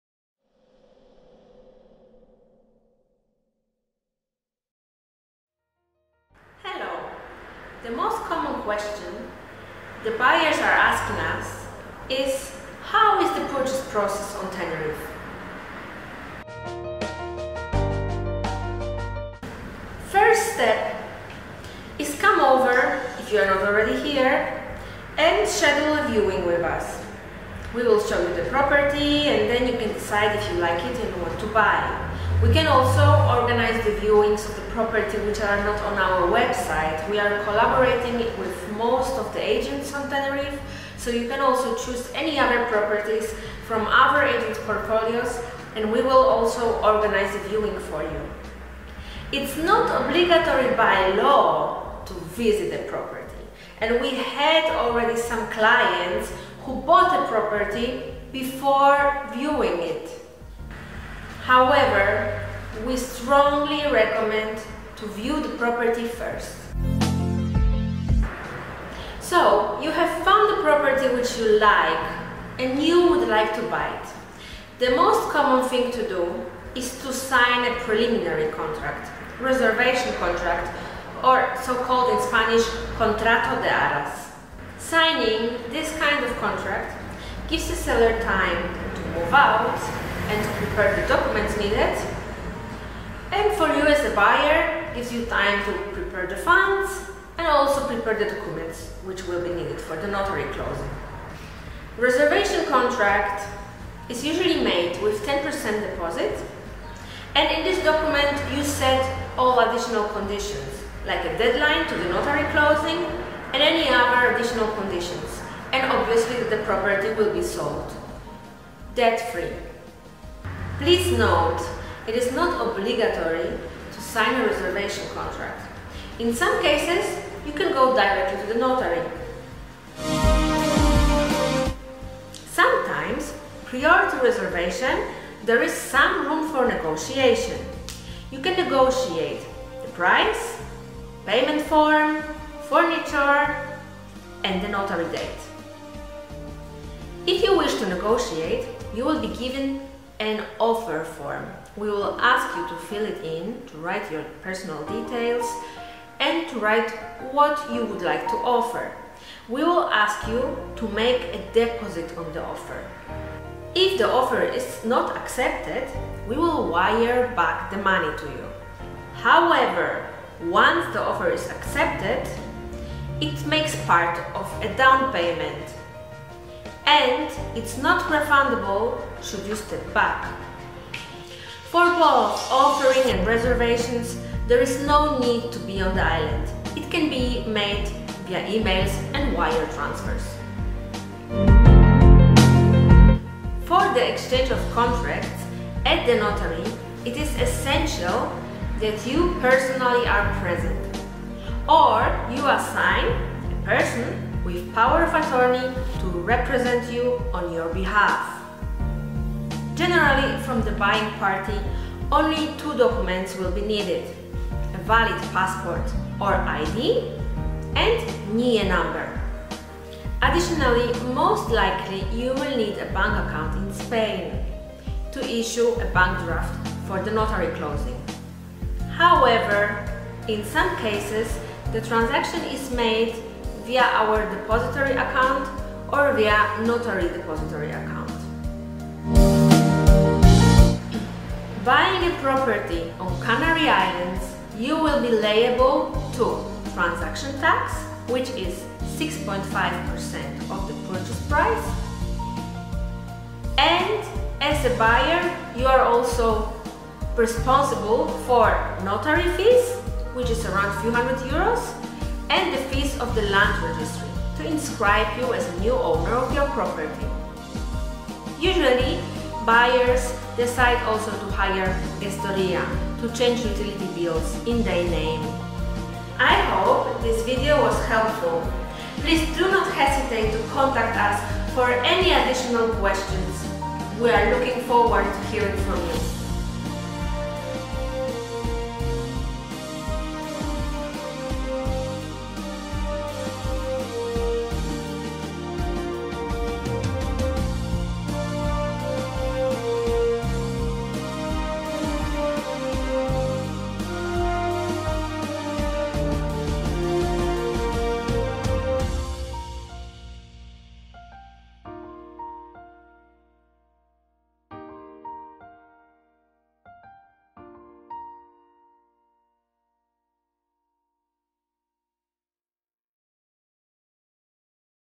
Buy a property on Tenerife (purchase procedure)
In this video I explain briefly the purchase procedure on buying a property on Tenerife

Please browse our website for more info and a great selection of the properties on our lovely eternal spring island.